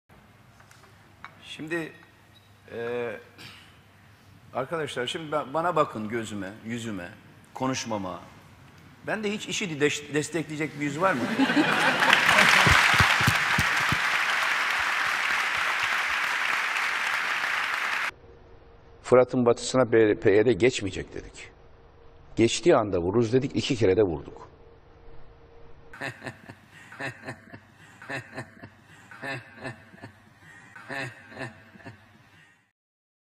IŞİD'e destek verecek yüz var mı sizce?
Gündüz, bende IŞİD'e destek verecek yüz var mı der, akşam ise Kürtler Fırat'ın batısına, yani IŞİD'in elinde olan bölgeye geçerse vururuz diye konuşur. Kürtlere ve dost halklara yaptığınız düşmalığı bir yana bırakalım, bundan daha iyi destek mi olur IŞİD'e ?